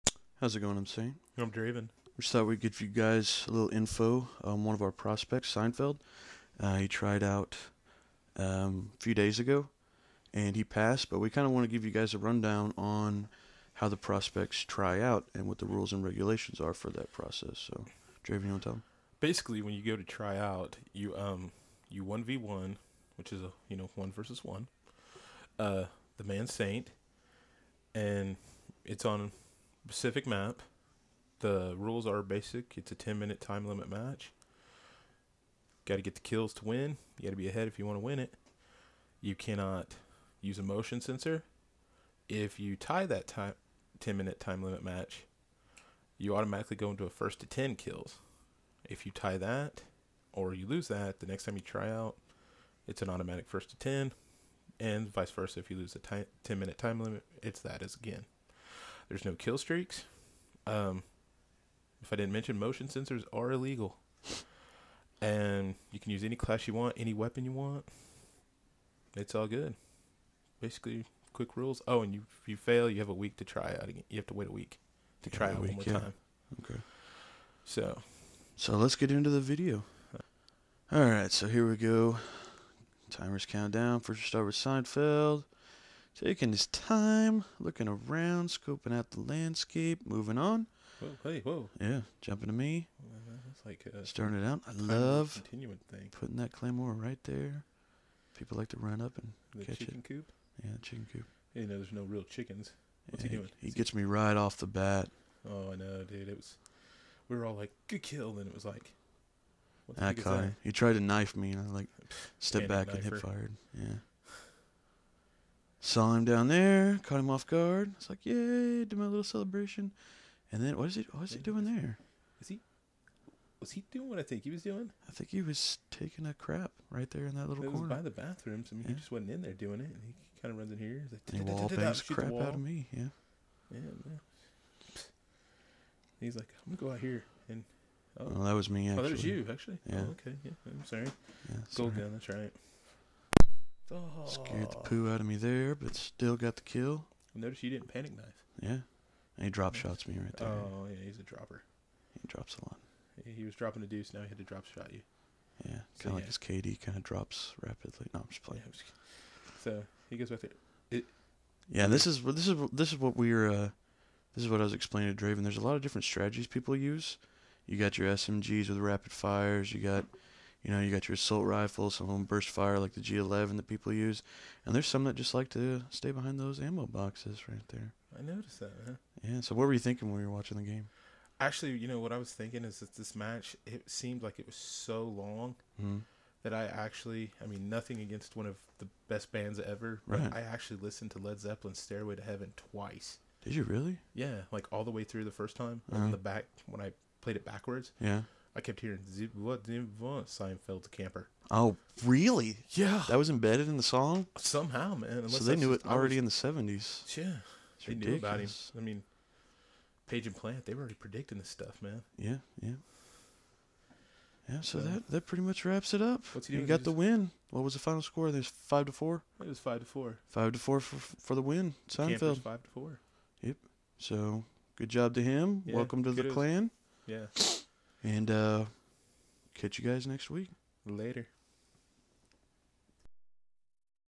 From "P" to "W" [Seinfeld's Ascension]
This is EsMw's sixth video on this channel. This is one of our prospects "SeinfeldSwagg91" tryout video. We hope you like it. We'd love to hear back from you. Twitter: @EsMwclan